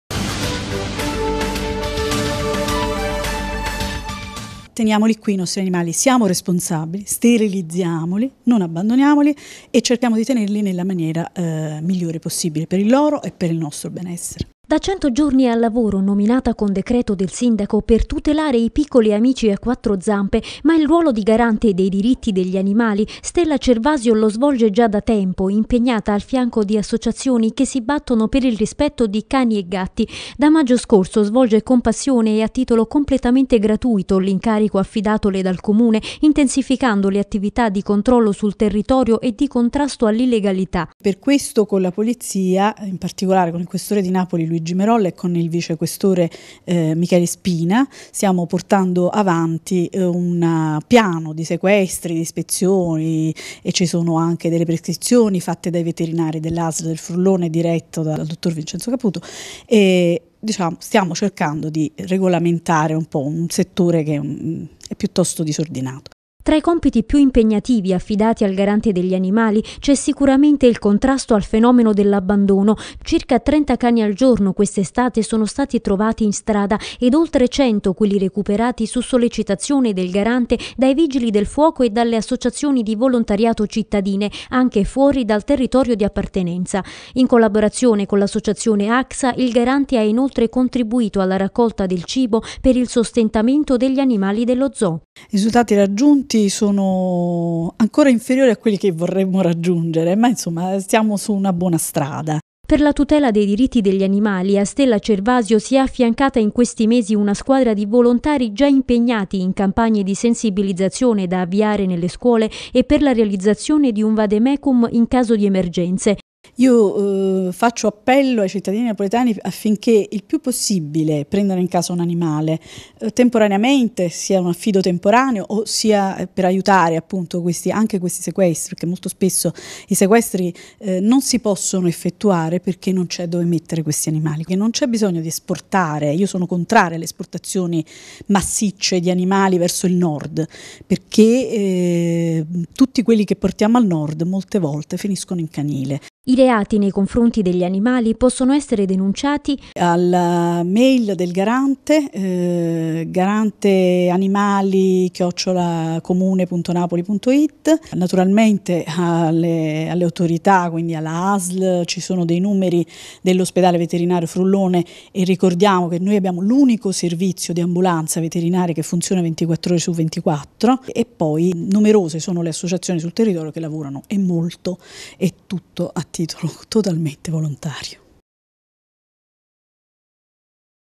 Tutela degli animali, proseguono le attività del Garante
A cento giorni dalla nomina a Garante degli Animali, Stella Cervasio fa il punto sulle attività svolte a tutela degli animali e dei loro diritti.